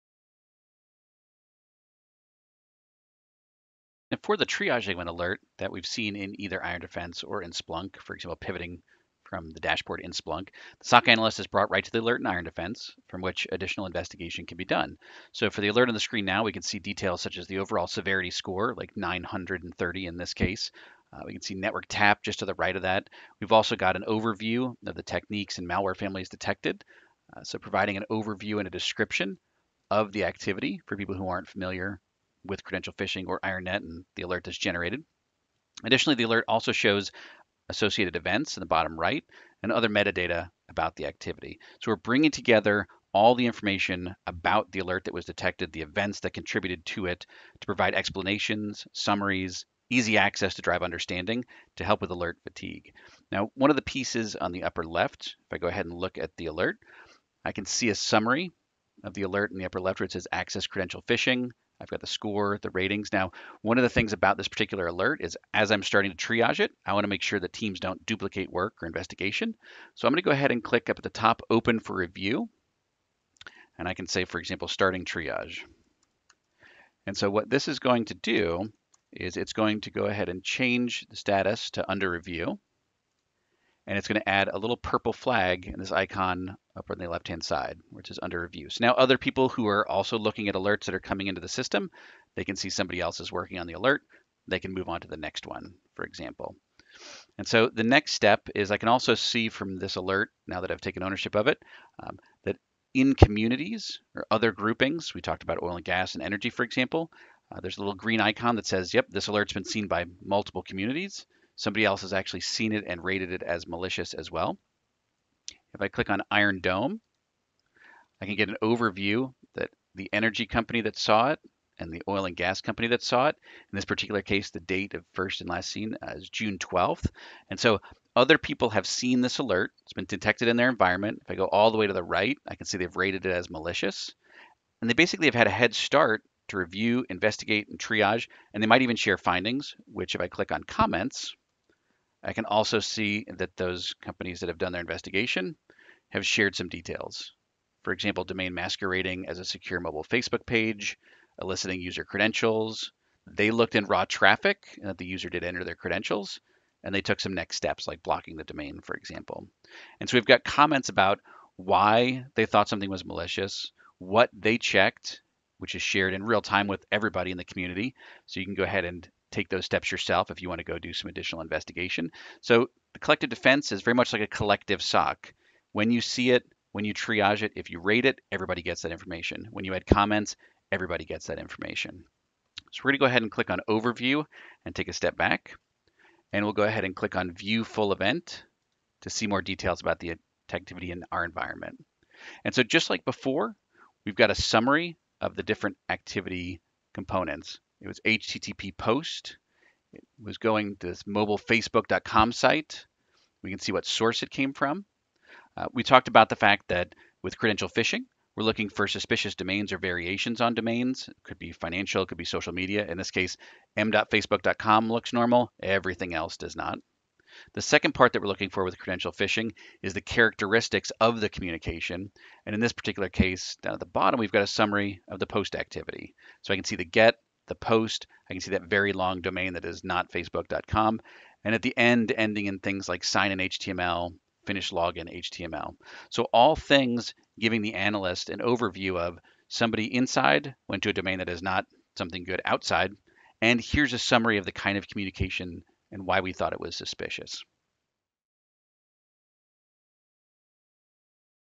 IronDefense Credential Phishing Investigation - Part 3
Part 3 of of this credential phishing demo series, walks through how a SOC analyst conducts an investigation of a detected credential phishing, how other SOC peers across the industry have rated and triaged similar alerts via IronDome Collective Defense sharing, and what the activity summary tells us about the alert.